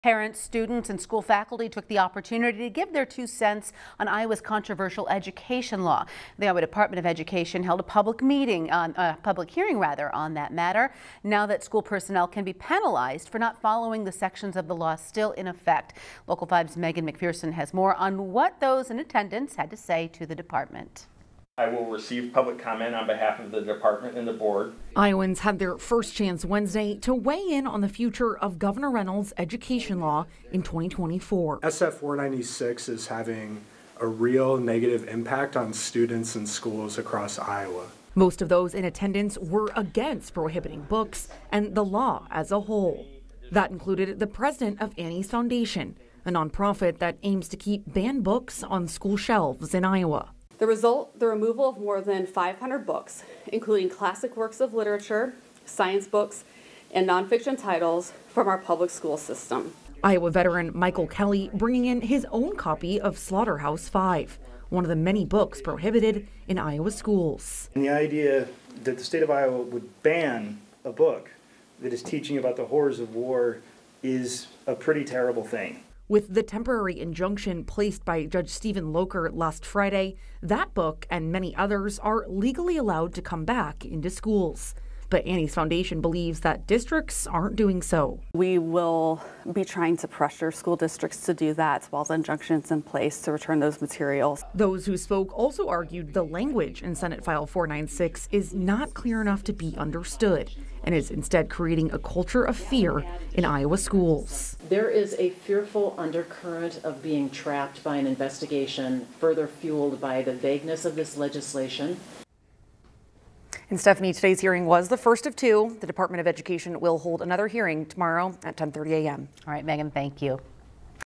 Iowans weigh in on controversial education law at public hearing
The Department of Education will hold another hearing tomorrow at 10:30 a.m.